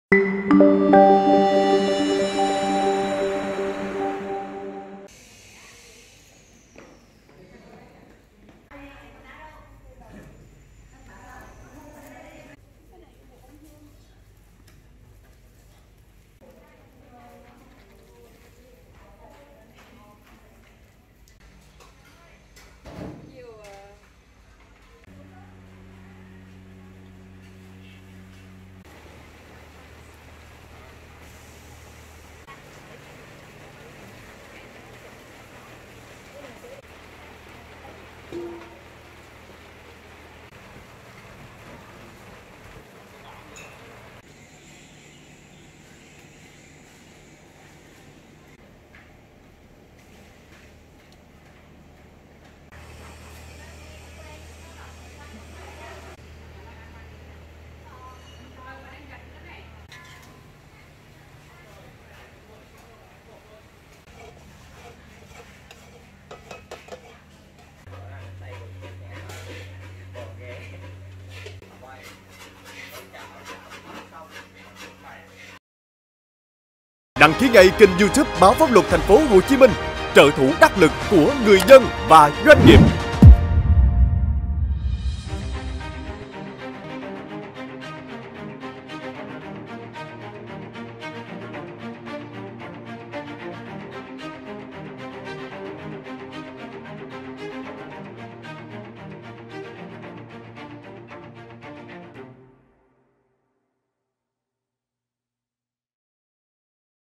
Vũng Tàu: Hàng ngàn chiếc bánh kèm muối mè gửi tặng miền Trung - PLO
Vũng Tàu: Hàng ngàn chiếc bánh kèm muối mè gửi tặng miền Trung
Đến chiều 21-10, hơn 1.000 bánh chưng bánh tét đã được hút chân không, đóng thùng cùng muối mè và chà bông gà chuẩn bị chuyển đi miền Trung vào sớm mai.